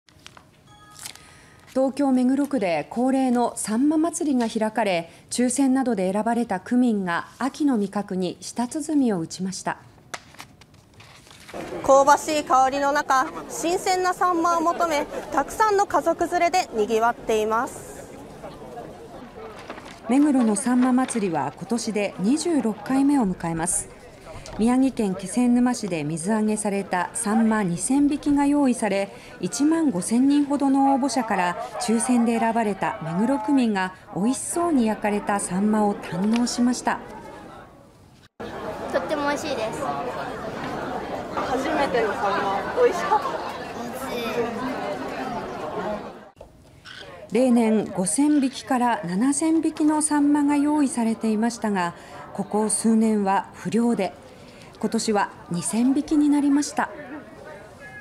「目黒のさんま祭」秋の味覚に舌鼓 不漁で今年は2000匹(2023年10月8日)
東京・目黒区で恒例の「さんま祭」が開かれ、抽選などで選ばれた区民が秋の味覚に舌鼓を打ちました。

　「目黒のさんま祭」は今年で26回目を迎えます。

　宮城県気仙沼市で水揚げされたサンマ2000匹が用意され、1万5000人ほどの応募者から抽選で選ばれた目黒区民がおいしそうに焼かれたサンマを堪能しました。

　食べた人：「とってもおいしいです」「（Q.初めてのサンマ、おいしいかったですか？）おいしい」

　例年、5000匹から7000匹のサンマが用意されていましたが、ここ数年は不漁で今年は2000匹になりました。